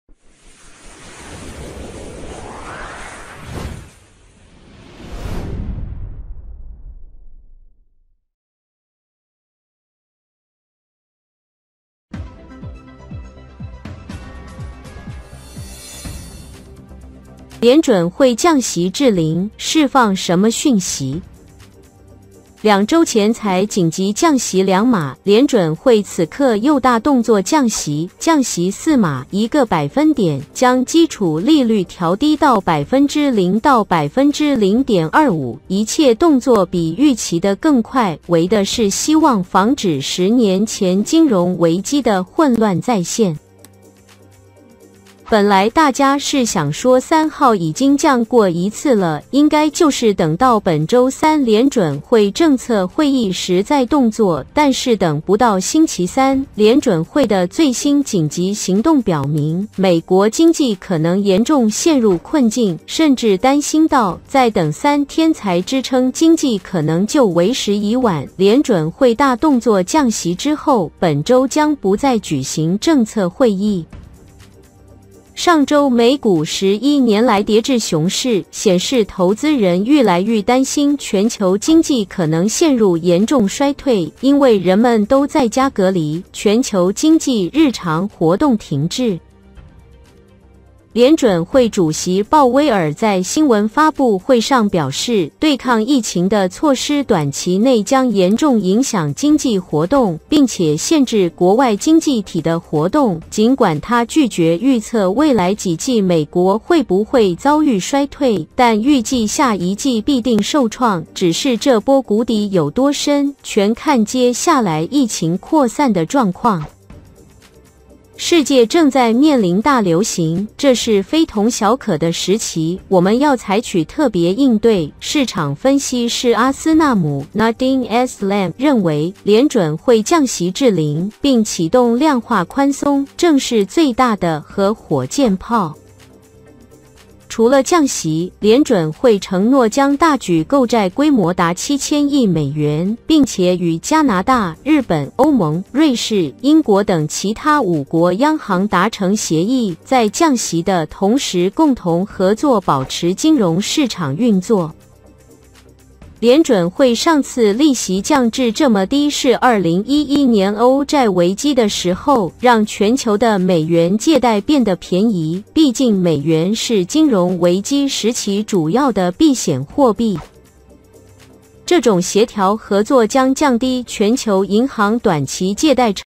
聯準會降息至零，釋放什麼訊息？-聯準會龜速降息? 川普揚言將要開除鮑爾! fed降息至零利率 道瓊期貨重挫千點｜主播鄧凱銘｜-更新新闻
兩週前才緊急降息兩碼，聯準會此刻又大動作降息，降息四碼（1個百分點），將基礎利率調低到0%-0.25%，一切動作比預期的更快，為的是希望防止10年前金融危機的混亂再現。